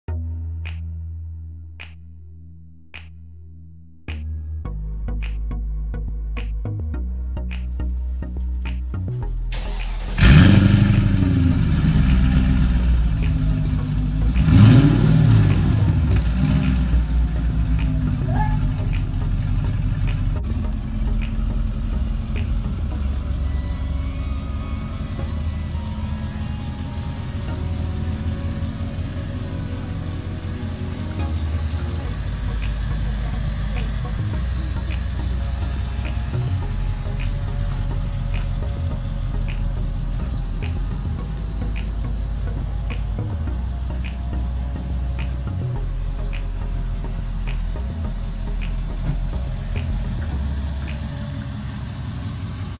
CLS 55 Warm Start up
2006 CLS 55 AMG that I'v owned for 11 years and still going strong